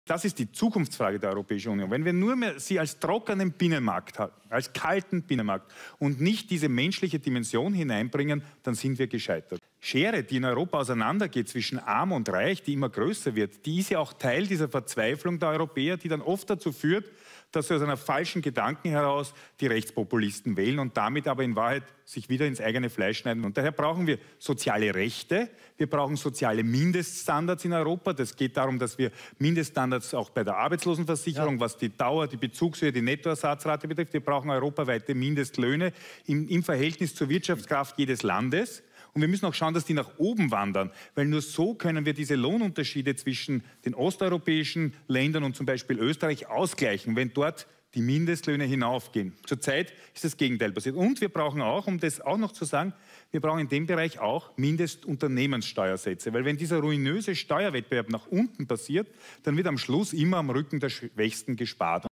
Andreas Schieder: Die EU darf nicht nur ein kalter Binnenmarkt sein!
Die EU darf nicht nur ein kalter Binnenmarkt sein. Wenn wir es nicht schaffen, eine menschliche Dimension mit hinein zu bringen, sind wir gescheitert.

Die Schere zwischen arm und reich geht immer weiter auseinander. Dies führt leider oft auch dazu, dass die Menschen auf Rechtspopulisten hereinfallen.

Wir brauchen soziale Rechte und Mindeststandards. Ebenso muss es gemeinsame Mindeststeuersätze für Unternehmen geben, um den Steuerwettbewerb nach unten zu bekämpfen. Nur so können wir verhindern, dass auf dem Rücken der Schwächsten gespart wird!
Ausschnitte aus der Diskussion der SpitzenkandidatInnen zur Europawahl. (ORF, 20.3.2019)